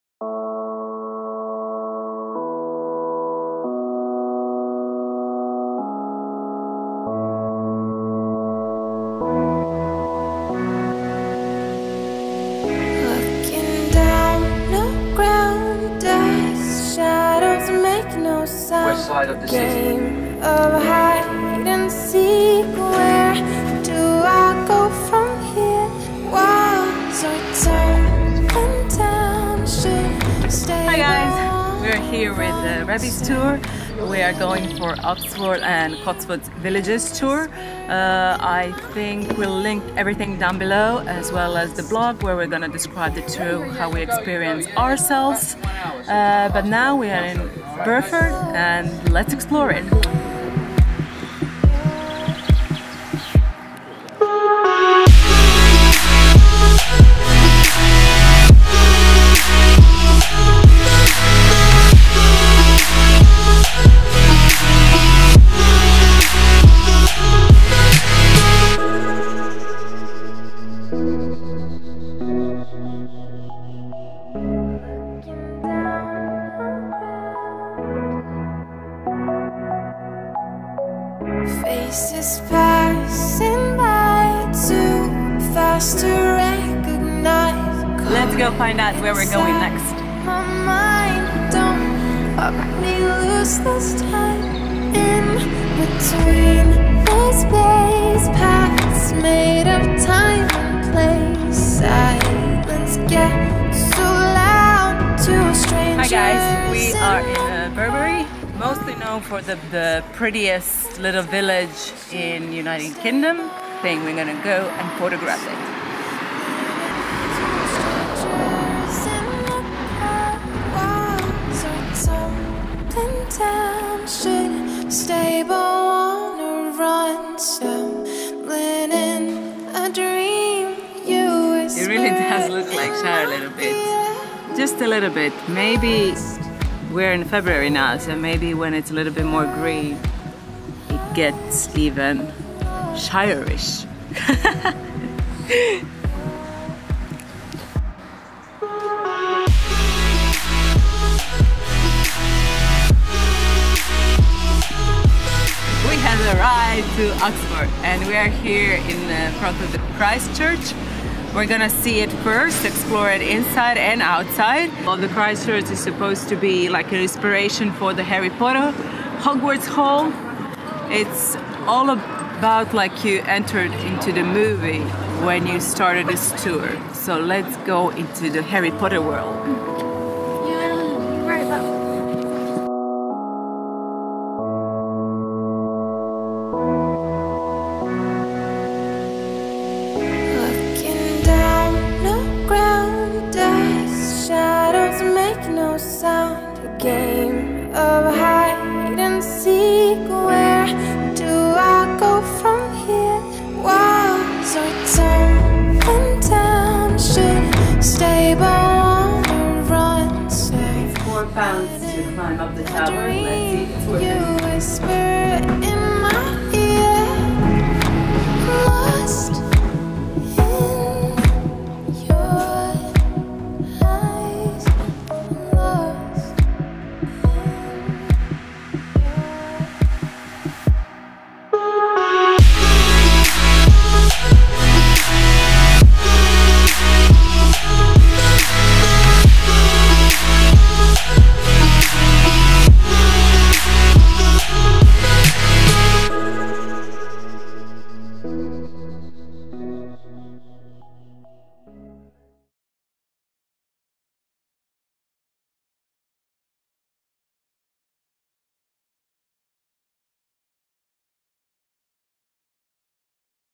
Oxford & Traditional Cotswold Villages London England Tour
Hello World!
If you have been following our Instagram, you know we visited London, UK. During our stay in London we have decide to go on Oxford & Traditional Cotswold Villages Tour with Rabbie's company.

Tour was amazing. In video you can see Bampton (Downton Abbey fans and history lovers will enjoy this traditional village’s honey coloured cottages.),  Bibury (This town arguably boasts the most charming village views in all of England.) and  amazing Oxford.